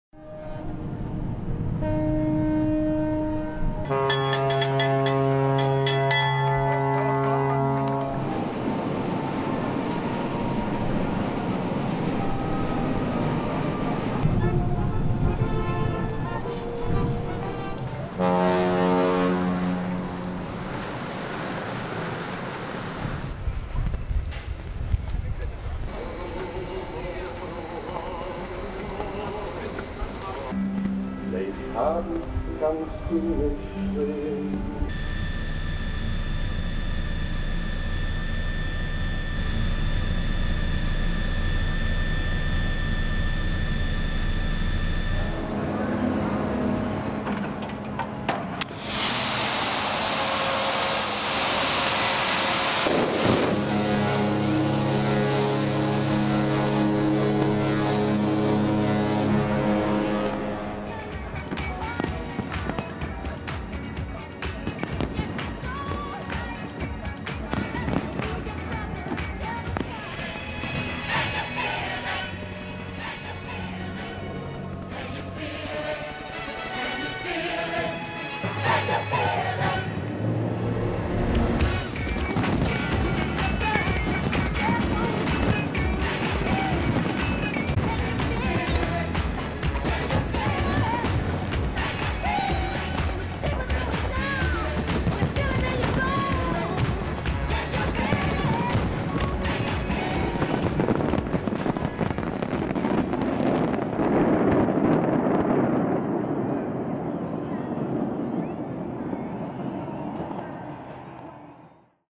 Hamburg : the world's greatest port festival
The Hanseatic city of Hamburg celebrates the 823rd Birthday of its port, from Friday, 11th May, to Sunday, 13th May. Some of the world's largest and most beautiful frigates and steamboats ships are expected.